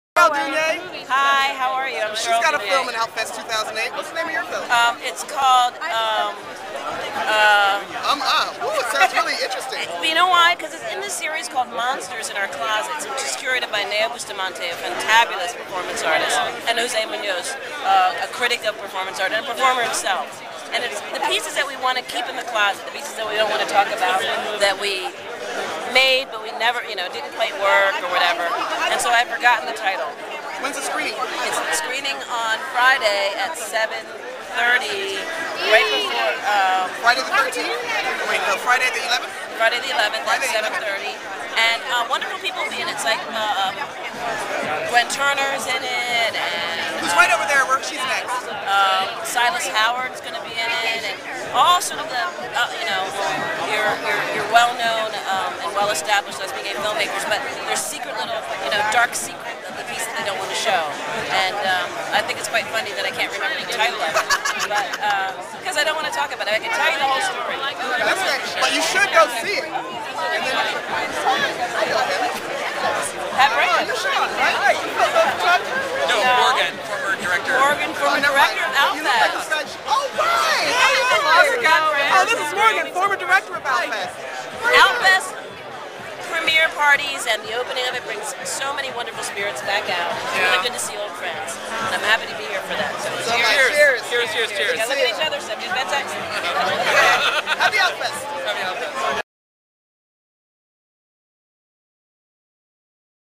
Outfest 2008 Interview with Cheryl Dunye
Pat Branch talks to Cheryl Dunye about monsters and Outfest.  Happy Outfest.